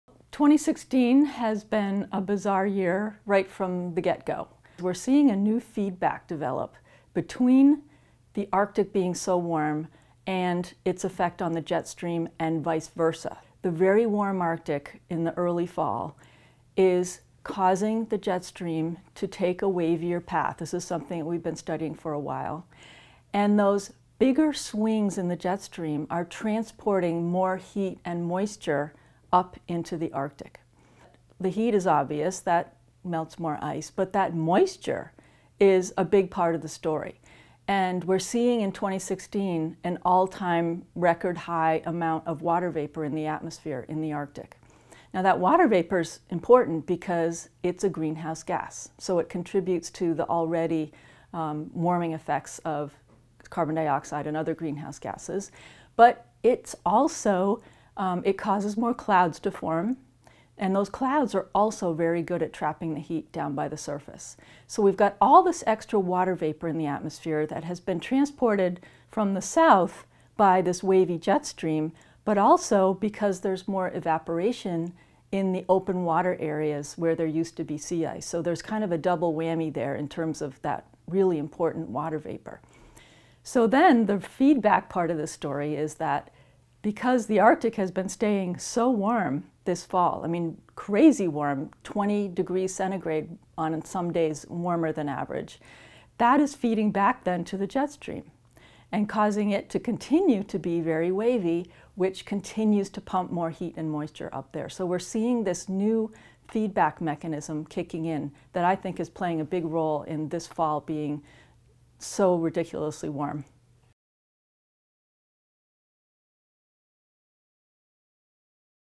Jennifer Francis: A New Arctic Feedback
A key idea in climate science is that the system contains multiple feedback loops, that can make warming worse.
For instance, as white, reflective ice is replaced by Dark, absorbing water - more of the sun's warmth is absorbed - a positive feedback. Here, Arctic expert Jennifer Francis describes a new, emerging feedback process, as the arctic warms and affects jet stream flow.